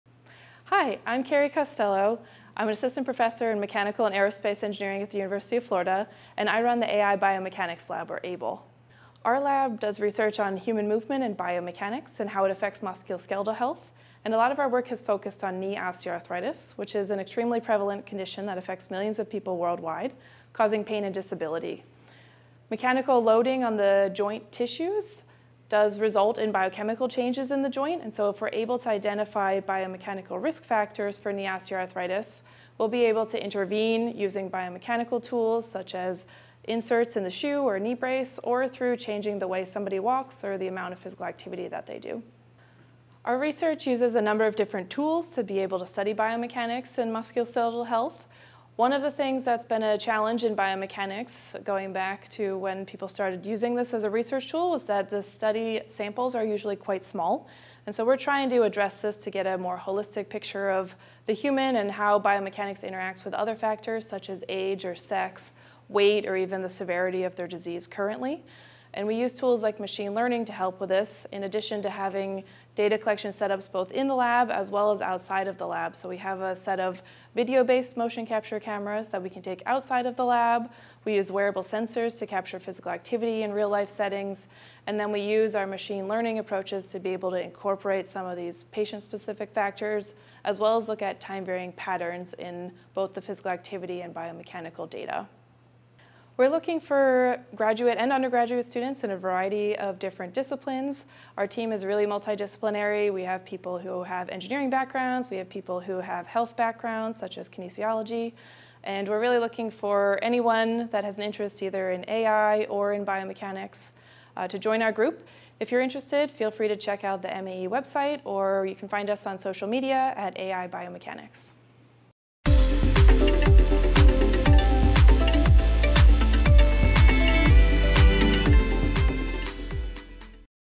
AI Biomechanics Lab (AIBL) - Kerry Costello
Dr. Costello provides an overview of the research conducted by the AI Biomechanics Lab (AIBL)